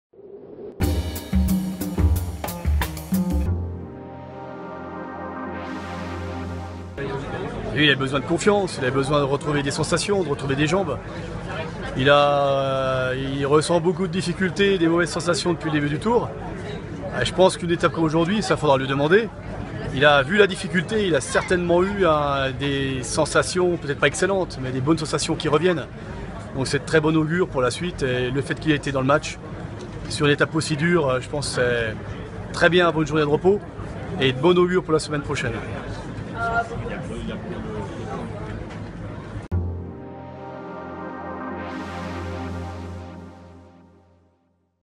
Tour de France 2017, 15ème étape - Frédéric Grappe trouve Thibaut Pinot rassurant
Tour de France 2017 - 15ème étape : Frédéric Grappe trouve la performance de Thibaut Pinot rassurante